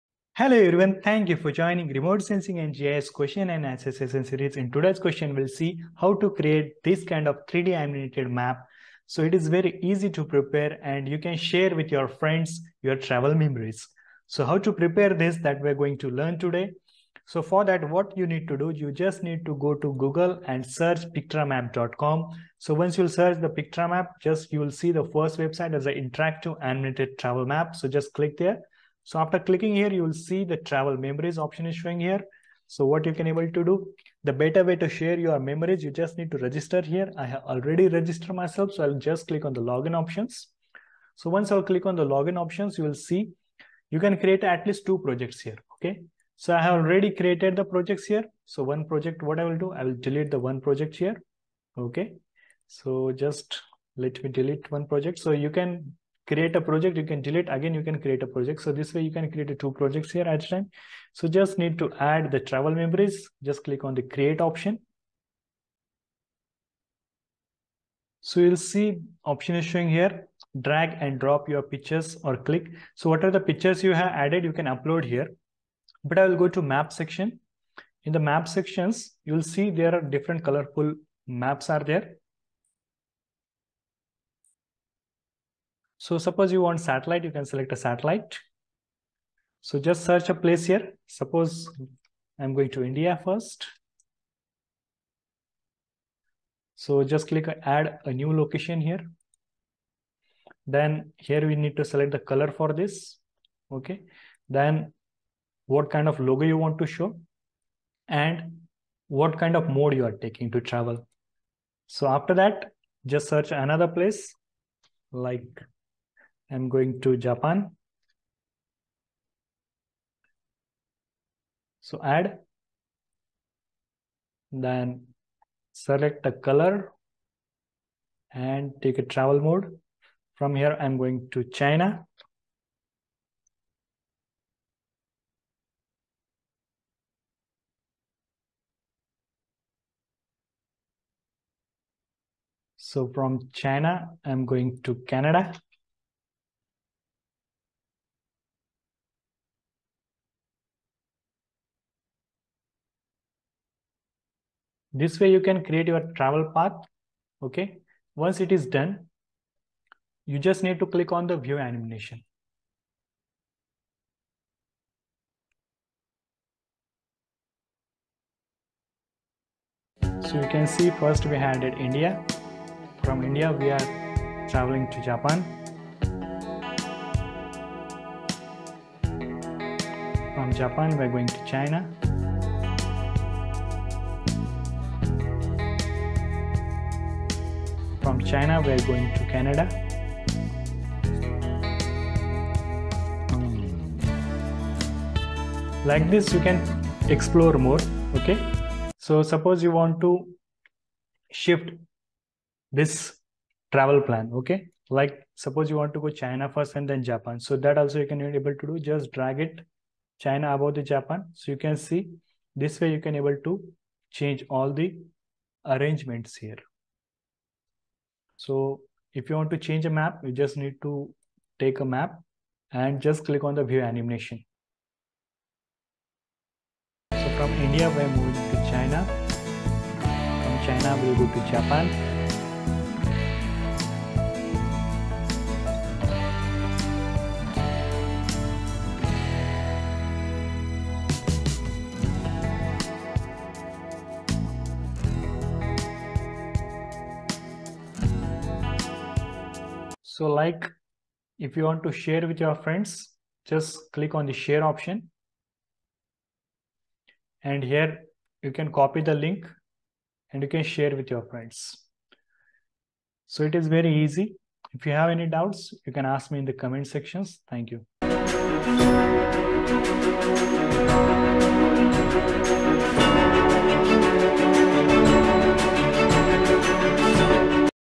RS & GIS QA24 Creating 3D animated travel memories maps || World Travel Map Animation
RS & GIS QA24 Creating 3D animated travel memories map

00:00 Intro
00:20 Pictramap search
01:07 Upload Photo
02:51 Animation
03:25 Chnage Order